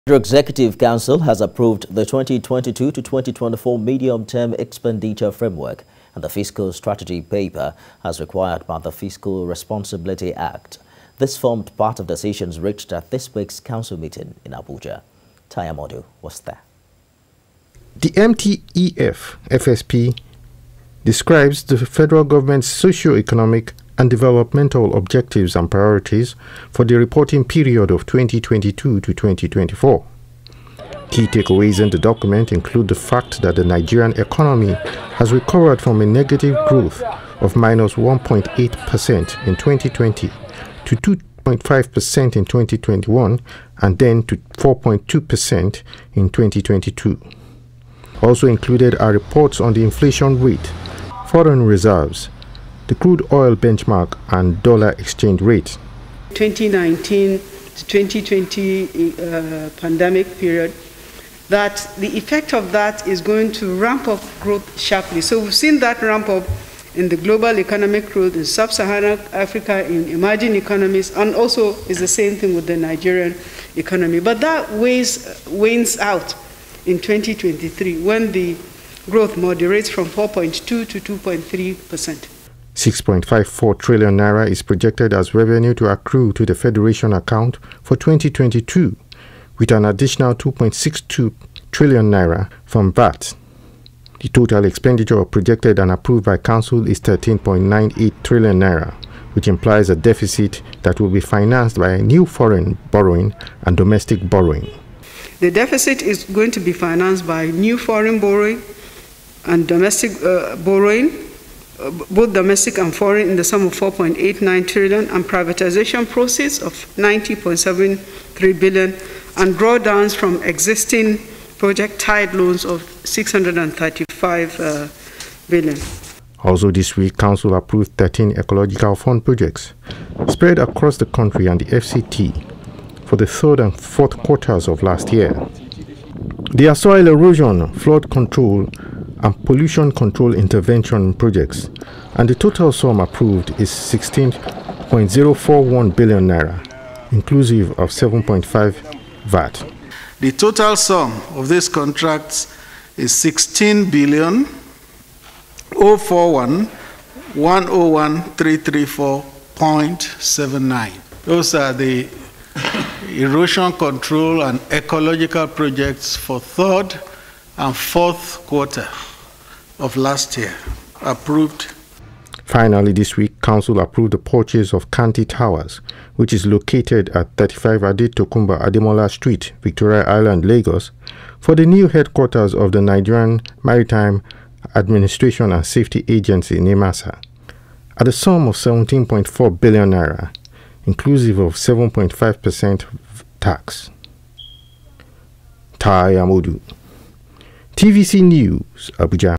FG Approves 2022 MTEF/FSP Documents As Required By Fiscal Responsibility Act
The Federal Executive Council has approved the 2022 to 2024 medium term expenditure framework and the fiscal strategy paper as required by the Fiscal Responsibility Act